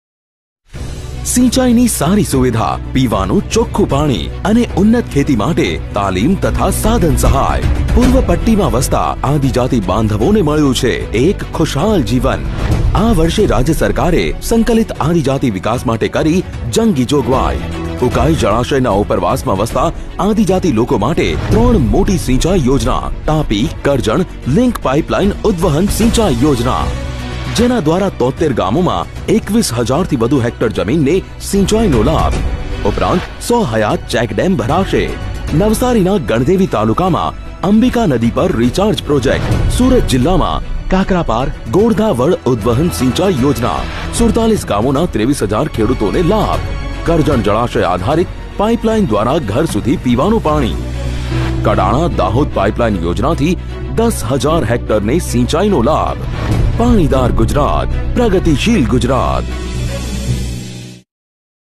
Gujarat Budget For Tribal Development
For the overall development of the tribal areas, the Gujarat Government has given special impetus to provide water for irrigation, potable water for drinking and skill training/tools for agrarian activities for the tribal areas in Budget 2019-20.